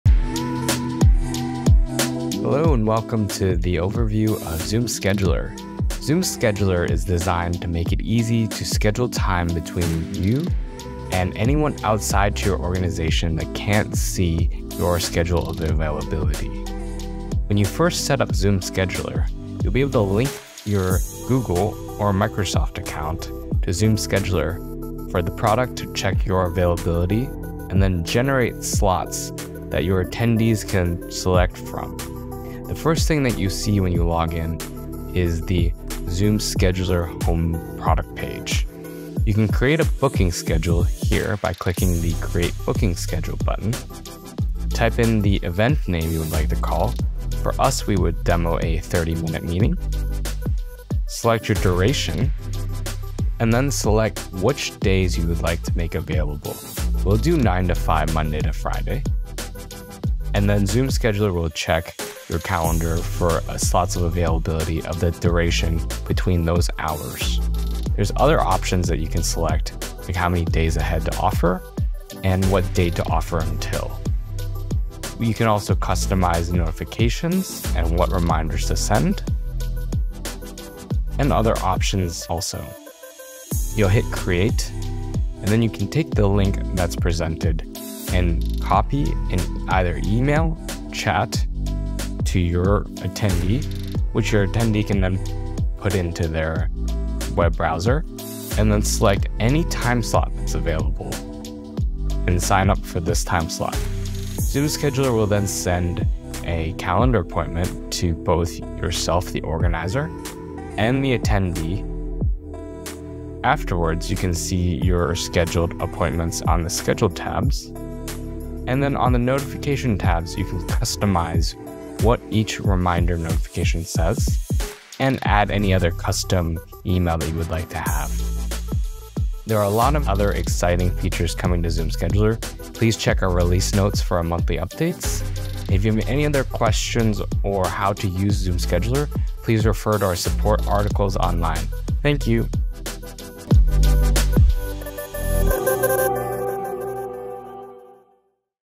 Zoom Scheduler demo
Ready to get started with Zoom Scheduler? Take a spin through the product in this short demo.